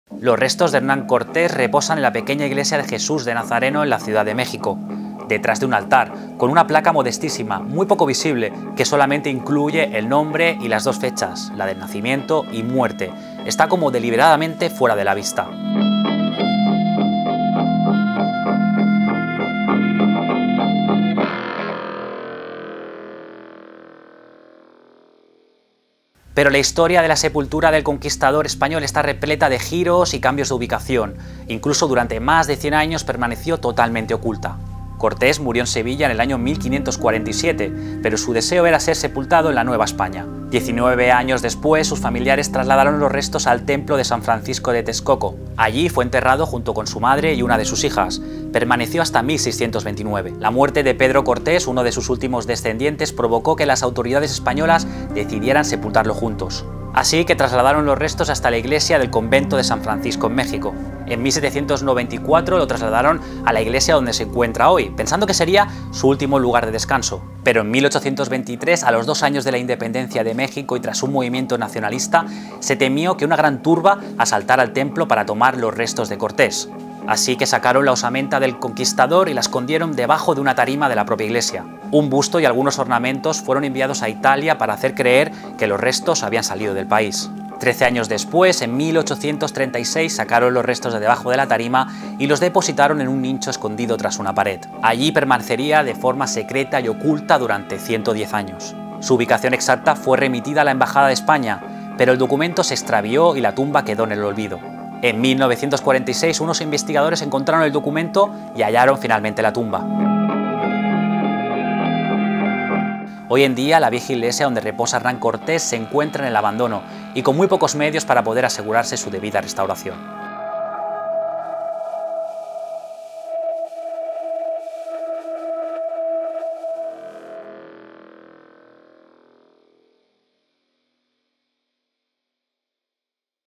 La enigmática historia de la tumba de Hernán Cortés
Los restos de Hernán Cortés reposan en la modesta iglesia de Jesús de Nazareno en la ciudad de México. Detrás de un altar, con una placa muy poco visible y que solamente incluye el nombre y las dos fechas, la del nacimiento y muerte. Está como deliberadamente fuera de la vista.

Pero la historia de la sepultura del conquistador español está repleta de giros y cambios de ubicación. Incluso durante más de 100 años permaneció totalmente oculta.